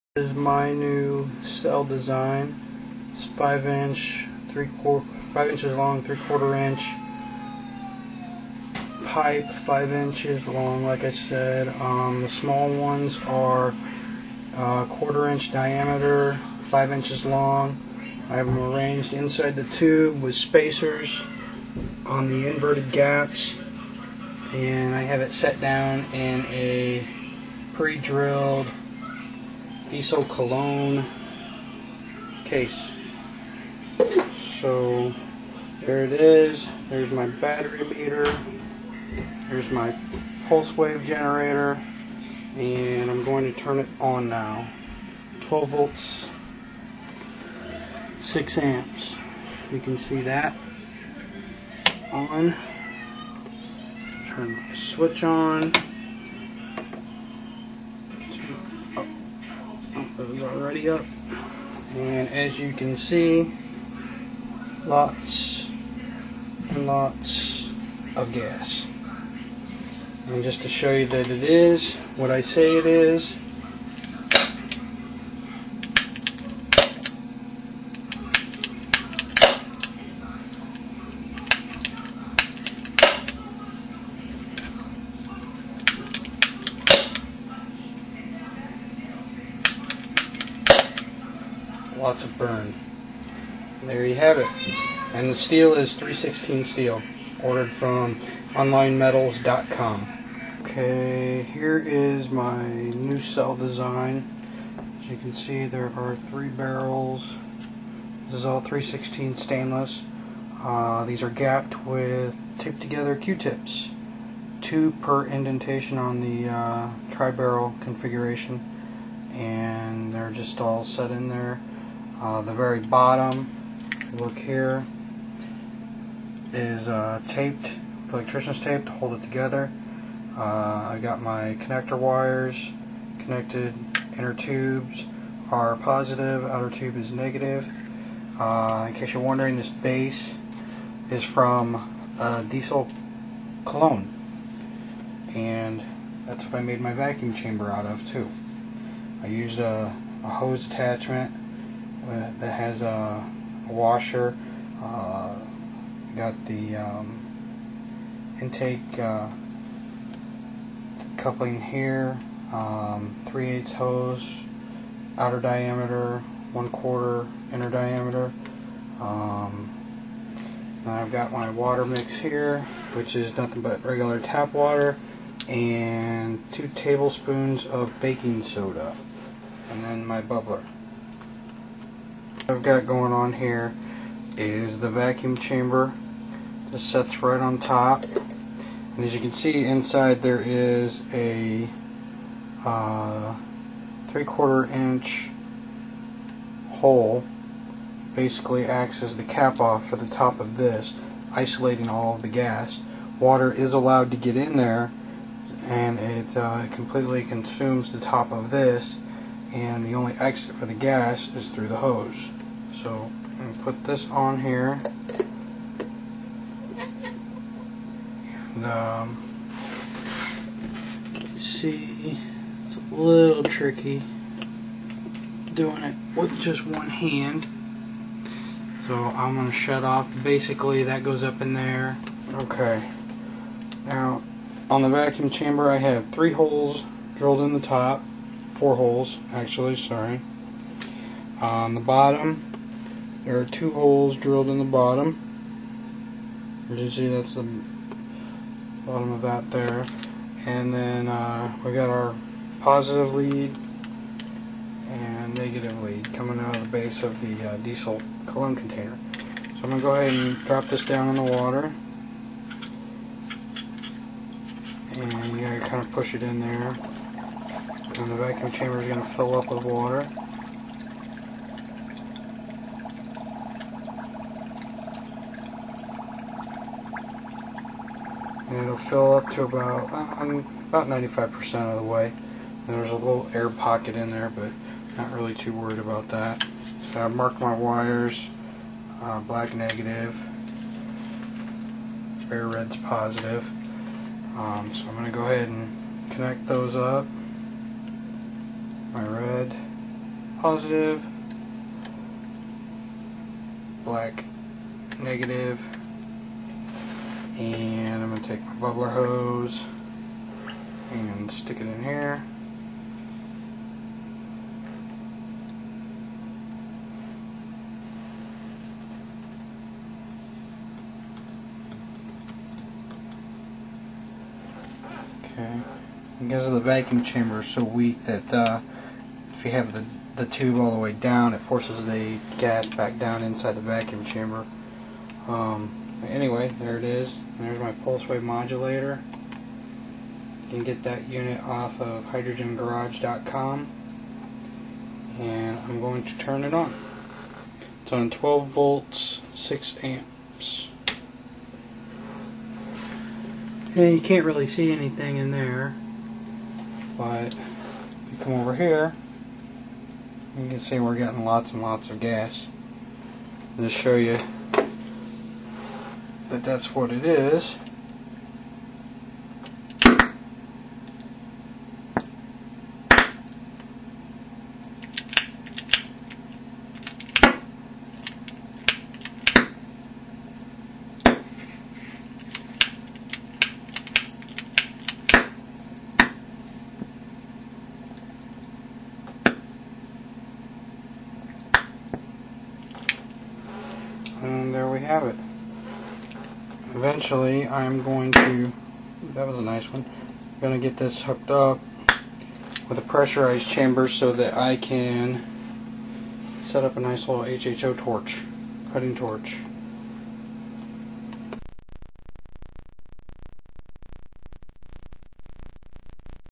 Hydrogen Reactor
A Hydrogen Reactor running on 12v/6amps using 316L Stainless Steel Tubing.